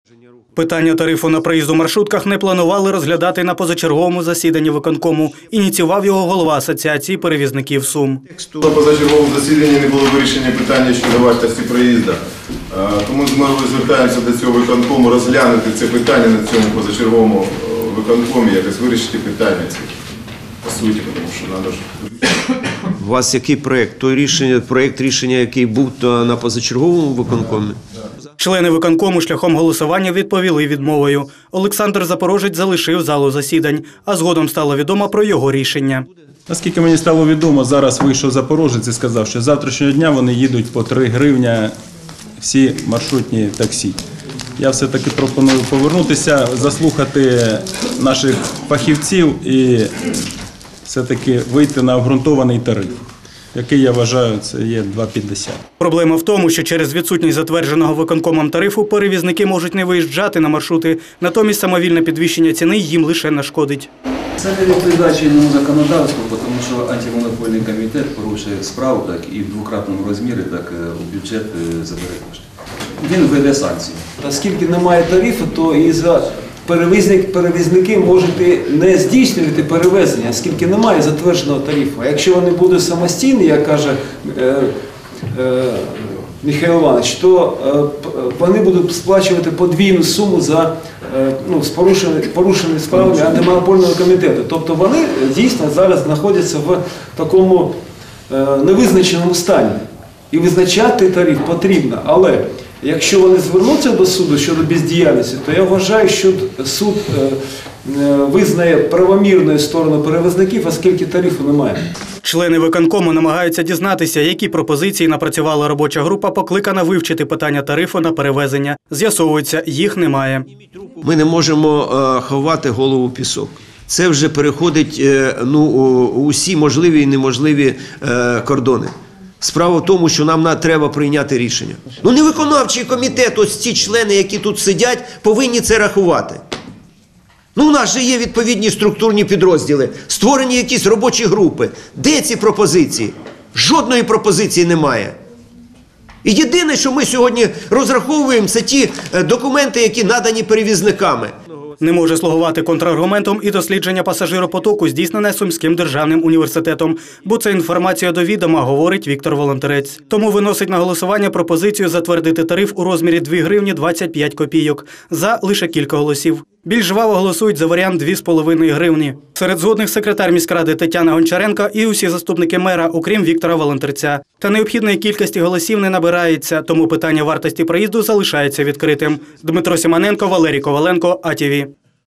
3 гривні може коштувати проїзд у сумських маршрутках завтра
3 гривні. Саме стільки  може коштувати проїзд у сумських маршрутках завтра. Так перевізники відреагували на небажання мерії підвищувати тариф. У відповідь на заяву, проблему спробували вирішити на позачерговому виконкомі.